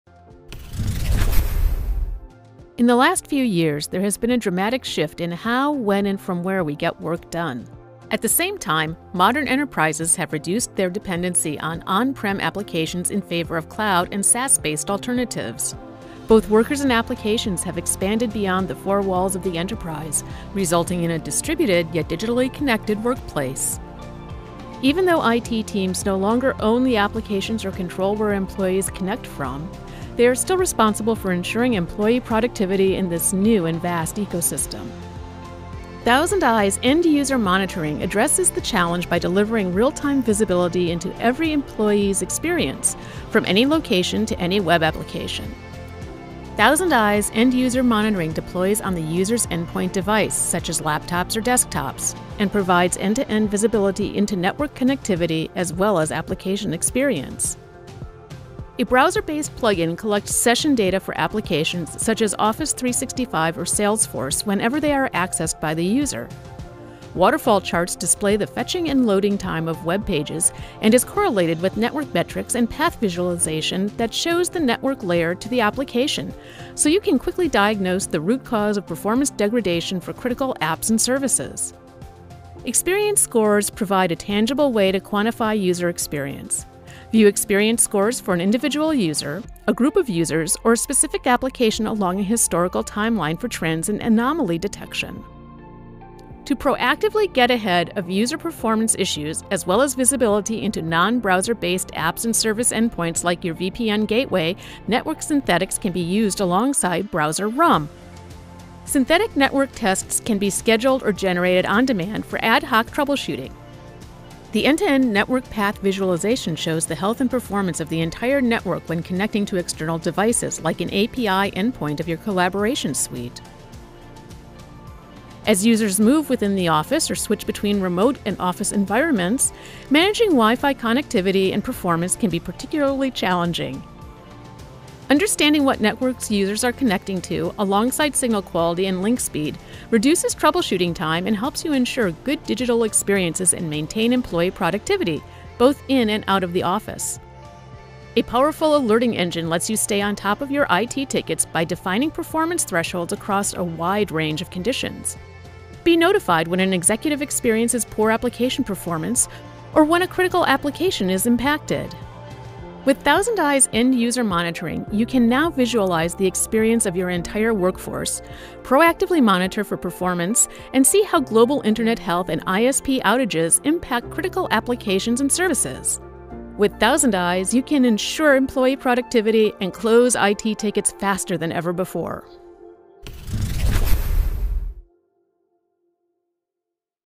End user Monitoring
Ensure employee productivity with ThousandEyes End-user Monitoring, delivering real-time visibility into every employee's digital experience from any location to any web application.